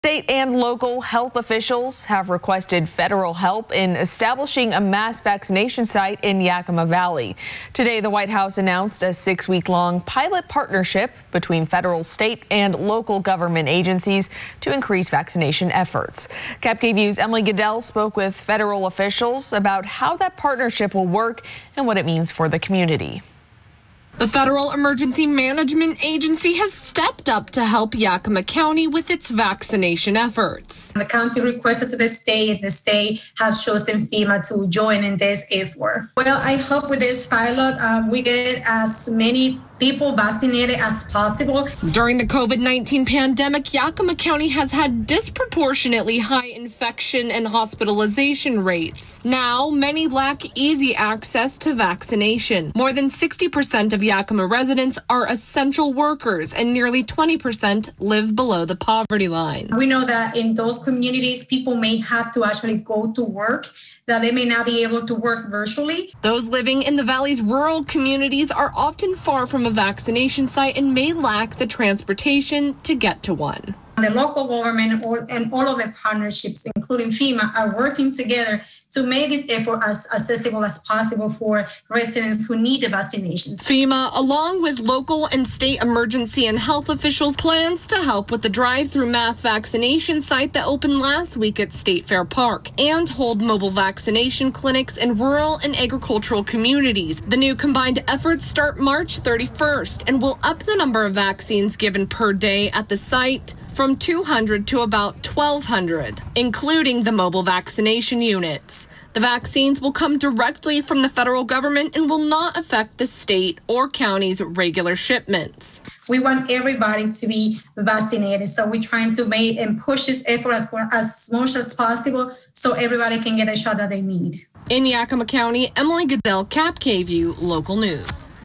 FEMA to help with Yakima mass vaccination site
Emily Goodell reports.